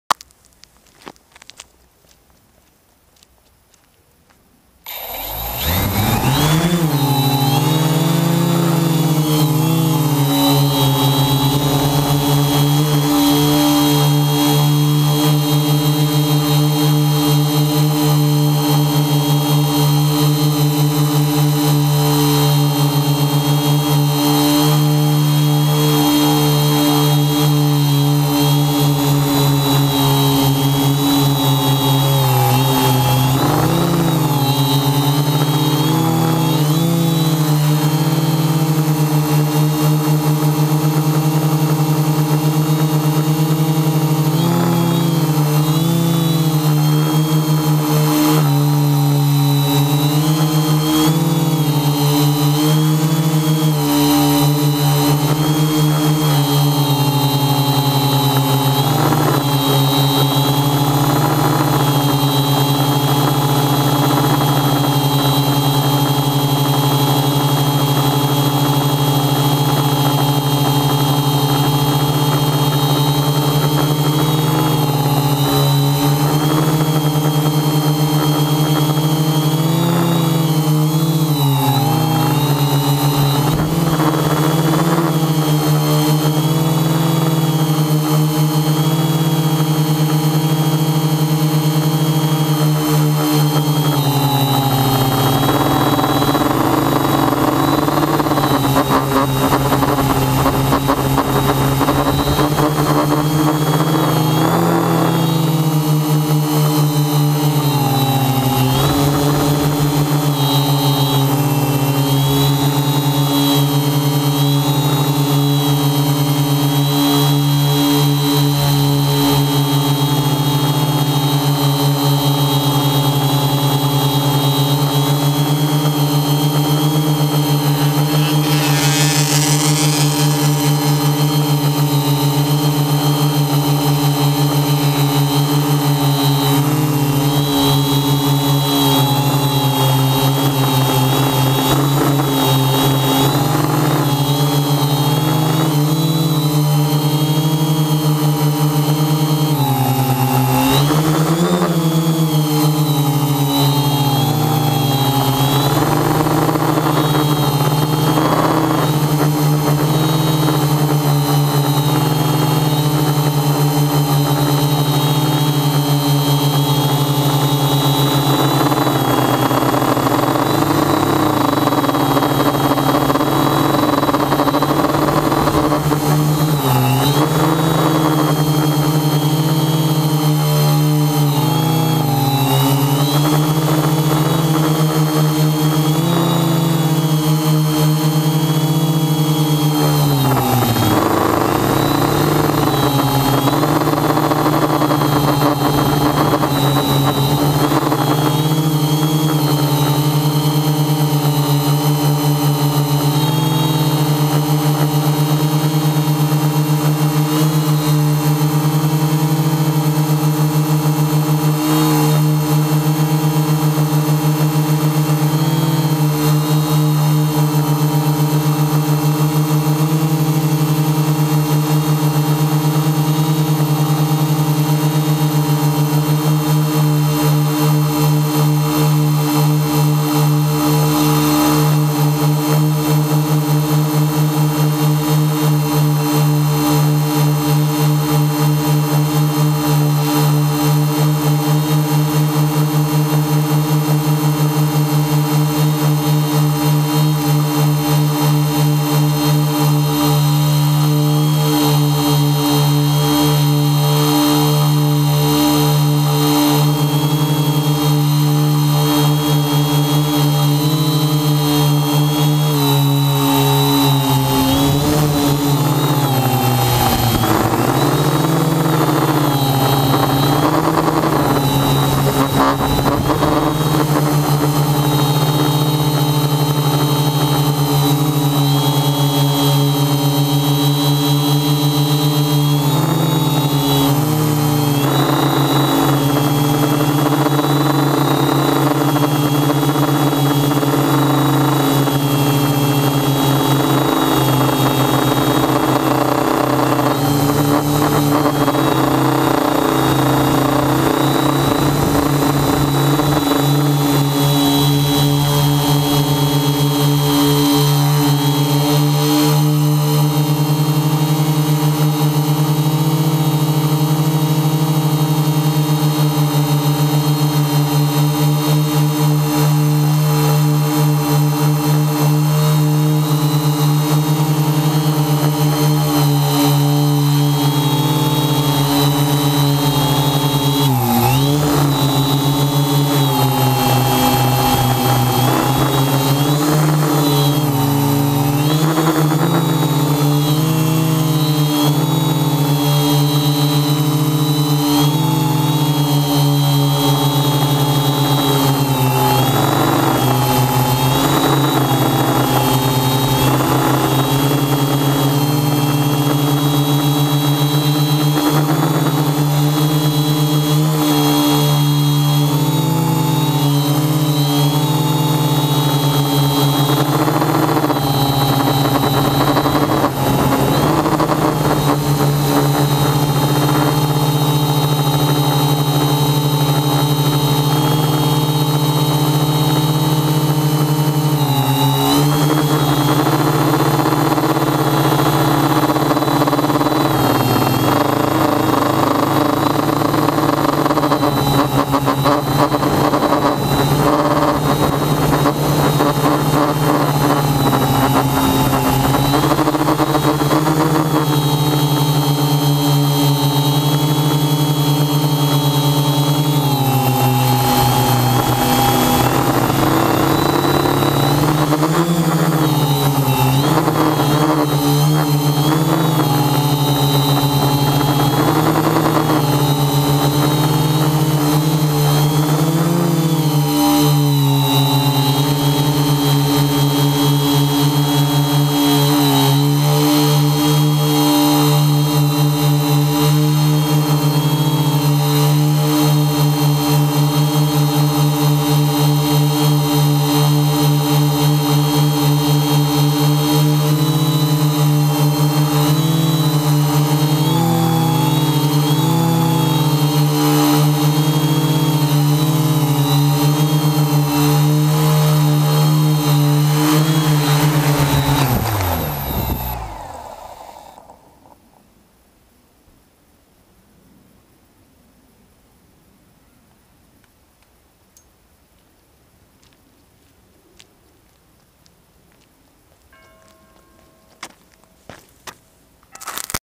FPV test
altro video in cui prendo ancora confidenza col volo fpv